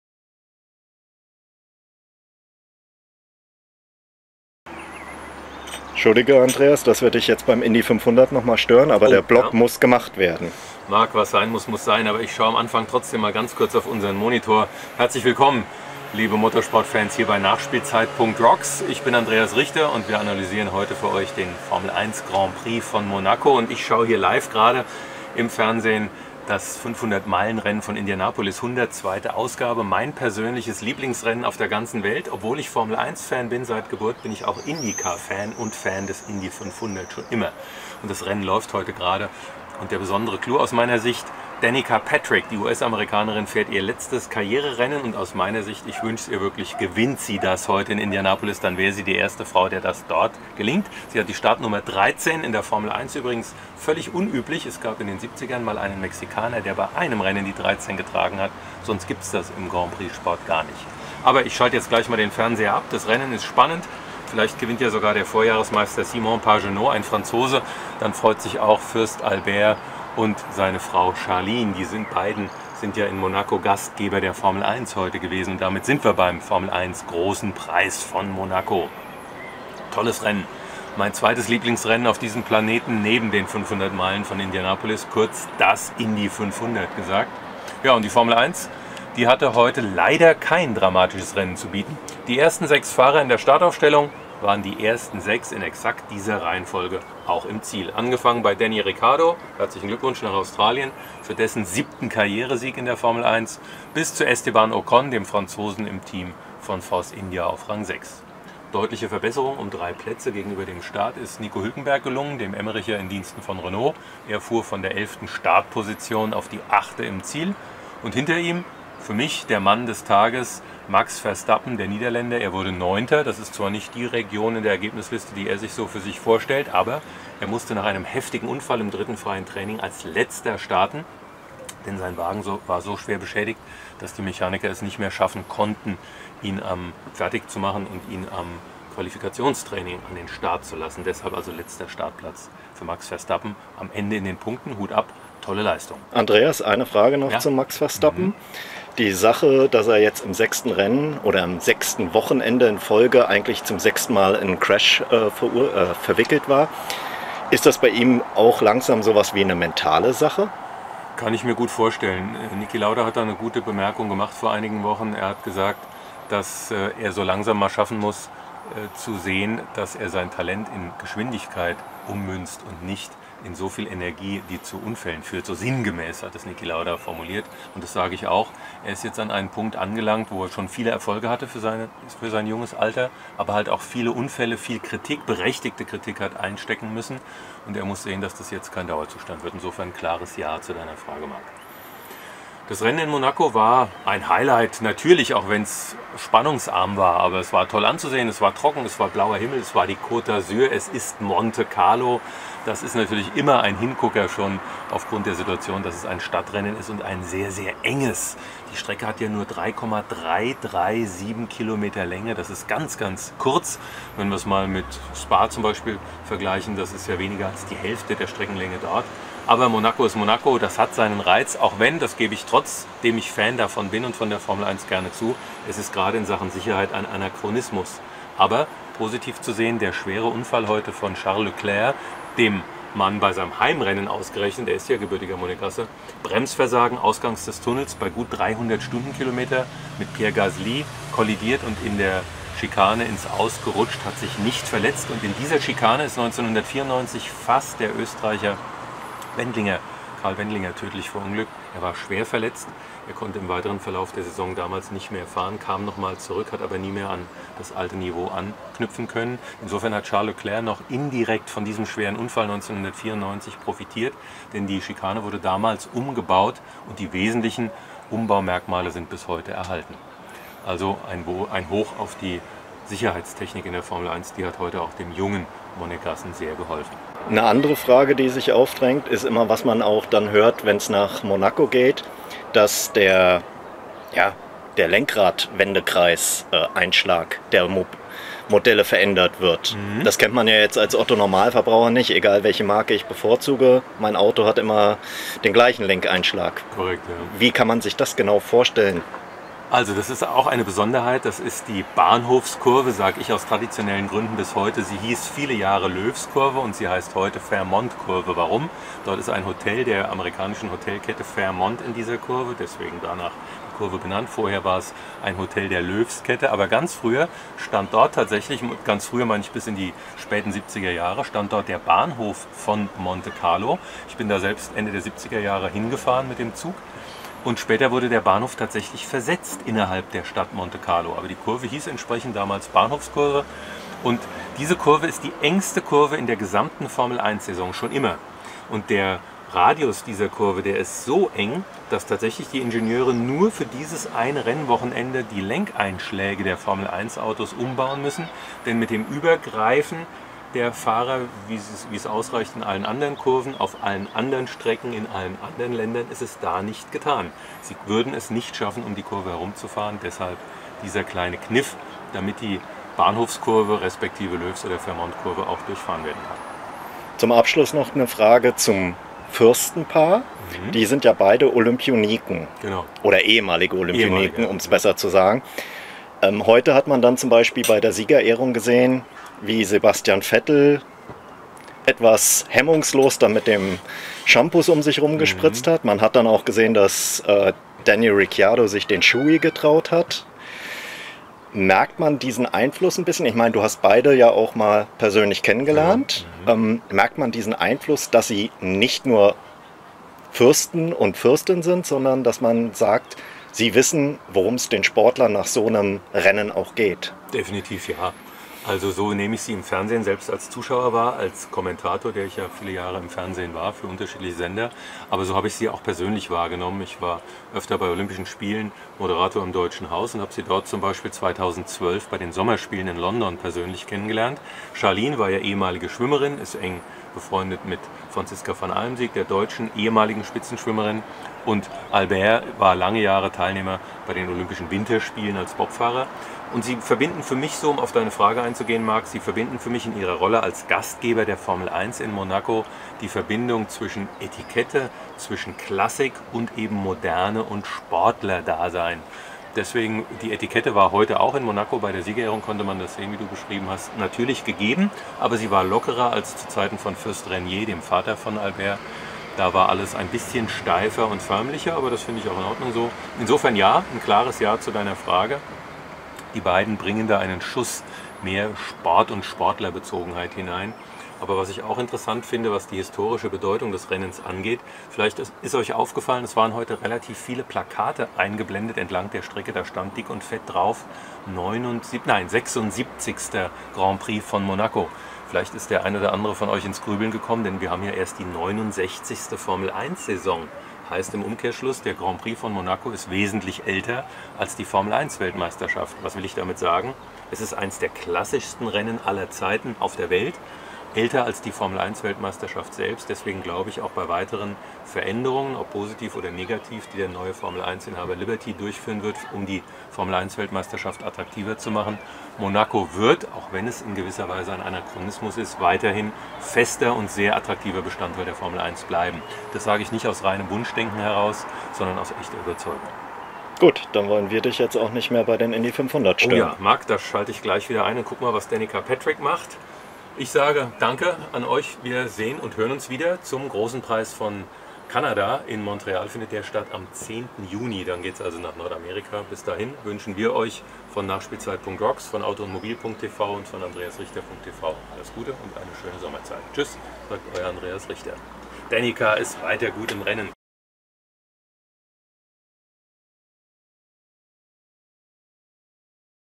a&mTV Formel1 Vlog 06 (Monaco 2018)
Dies ist der offizielle Formel1-Blog von Das nachspielzeit-Team wird bei diesem Vlog von der Redaktion von (a&mTV) unterstützt. Diese Ausgabe dreht sich um das Rennen in Monaco
1. Daniel Ricciardo (Red Bull)
2. Sebastian Vettel (Ferrari)
3. Lewis Hamilton (Mercedes)

Mann des Rennens: Max Verstappen

Im Fokus von Moderator und Kommentator Andreas Richter: Die Besonderheiten des Rennens im Fürstentum.

handelt es sich um eine Webseite mit täglichen Inhalten rund um die Mobilität. Im Bereich Motorsport unterstützt das a&mTV-Team exklusiv nachspielzeit.rocks.